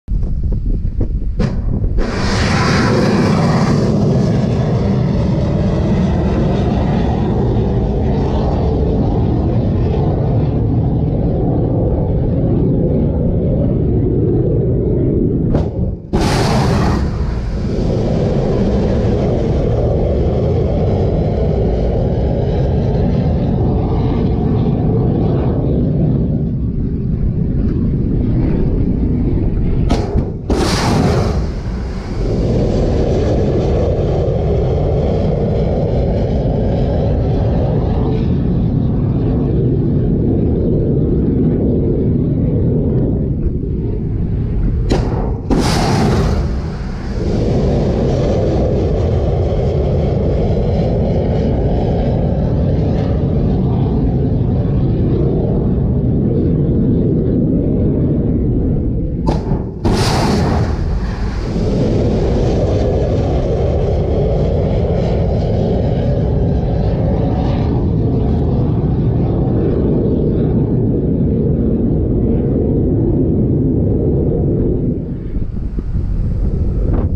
US strikes in Syria launched from USS Porter -3
MEDITERRANEAN SEA (April 7, 2017) The guided-missile destroyer USS Porter (DDG 78) conducts strike operations while in the Mediterranean Sea. Porter, forward-deployed to Rota, Spain, is conducting naval operations in the U.S. 6th Fleet area of operations in support of U.S. national security interests in Europe. (U.S. Navy video by Mass Communication Specialist 3rd Class Ford Williams/Released)